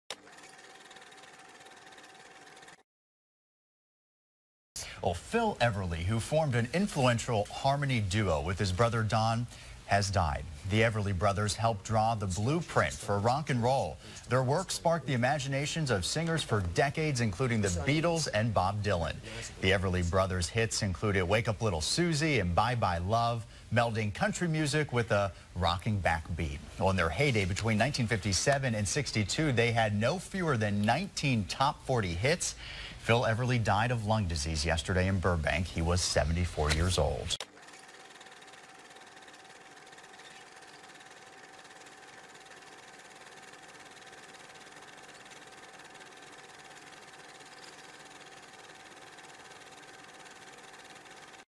Phil Everly: News Report of His Death - January 3, 2014
Everly Brother, Phil Everly, has died.  He was 74 years old.

Phillip Everly (January 19, 1939 – January 3, 2014) was an American musician, who was one half of the duo The Everly Brothers alongside his older brother Don.